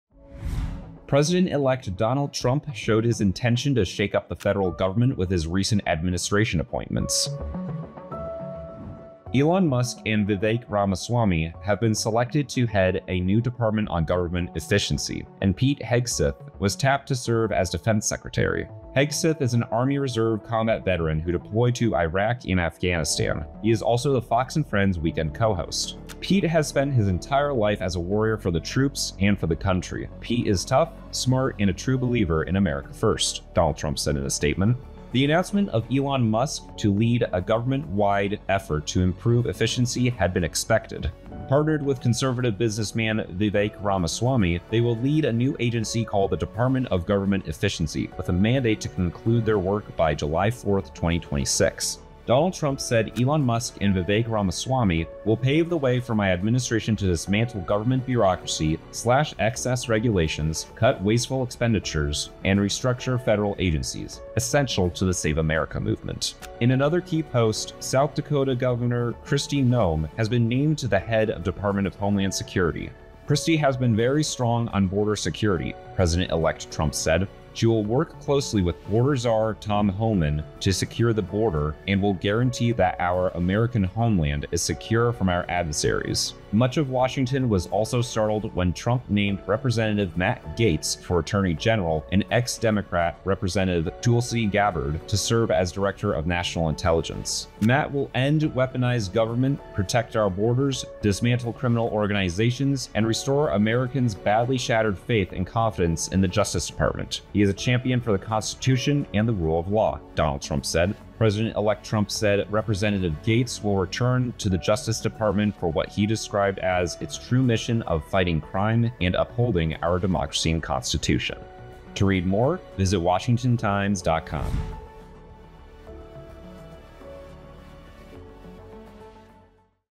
Donald Trump's administration appointments
President-elect Donald Trump showed his intention to shake up the federal government with his recent administration appointments.